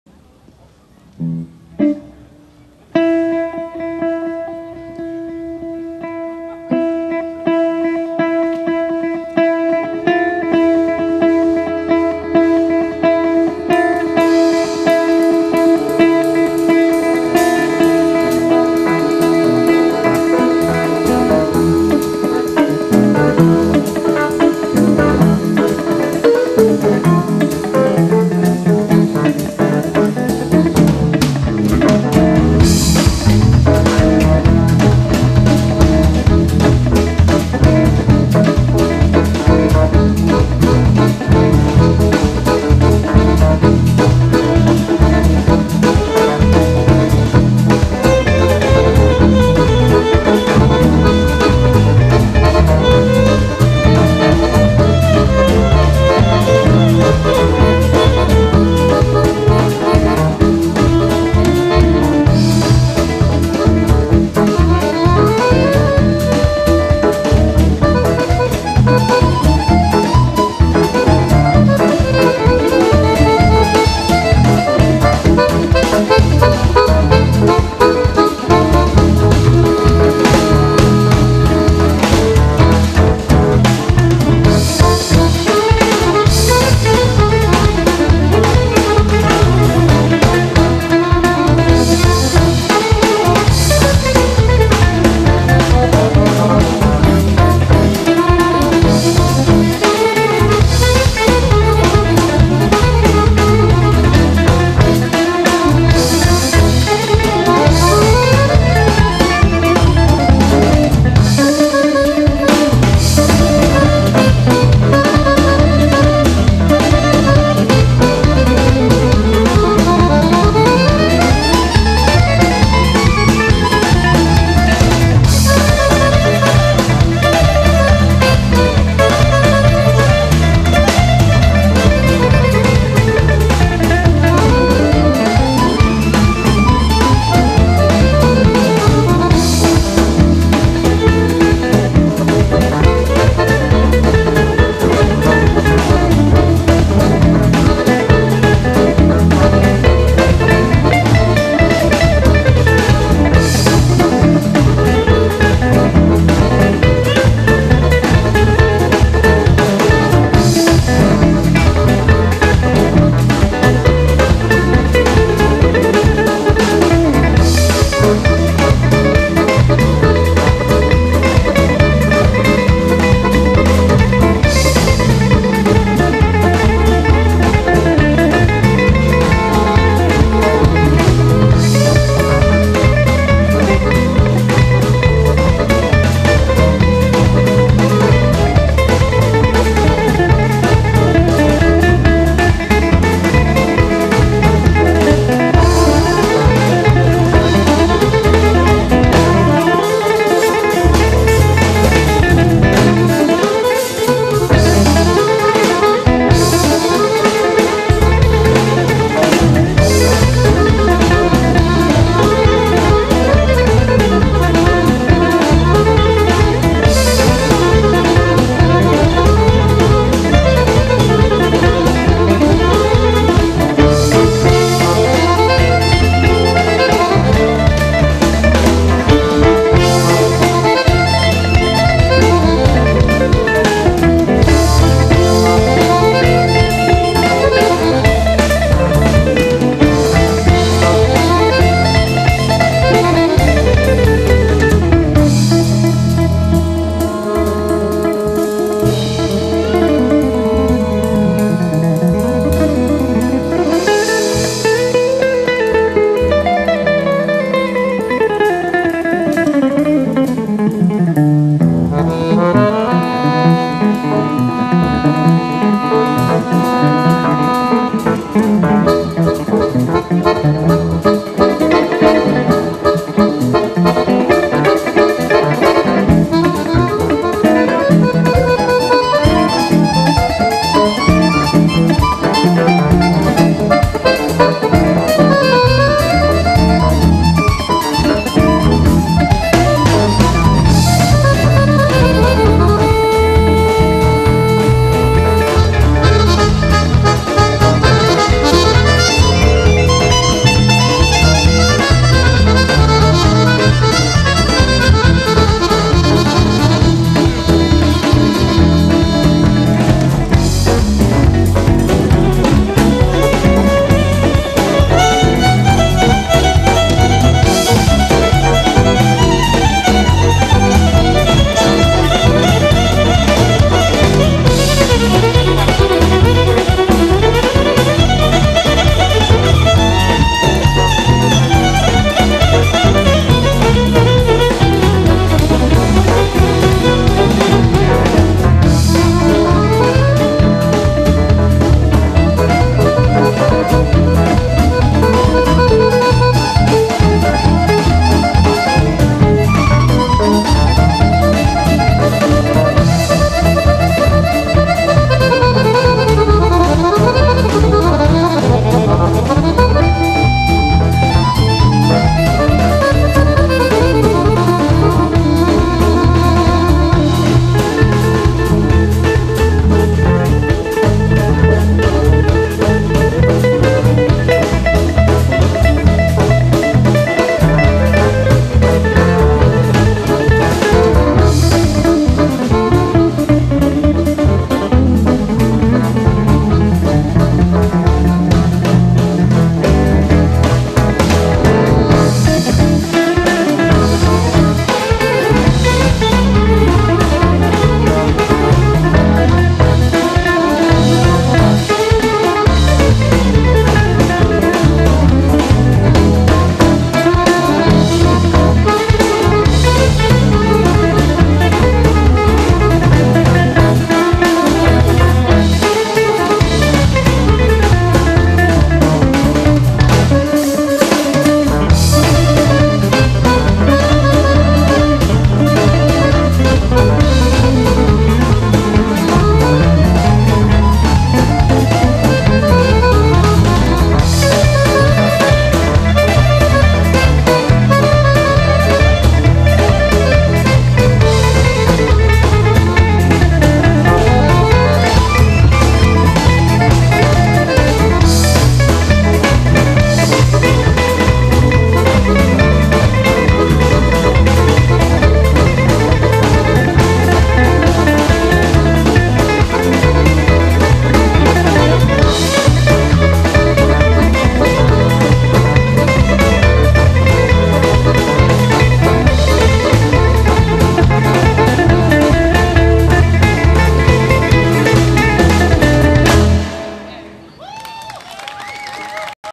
Nista Corbasto - Nisville 2012 - 02
Nista Corbasto Orchestra live at Nisville Open Stage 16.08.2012.

Nikola Savic - Accordion / Harmonika
Srdjan Miskovic - Violin / Violina
Vlade Kapetanovic - Organ and Bass
Ramon Hamel - Drums / Bubnjevi
Dejan Krsmanovic - Guitar / Gitara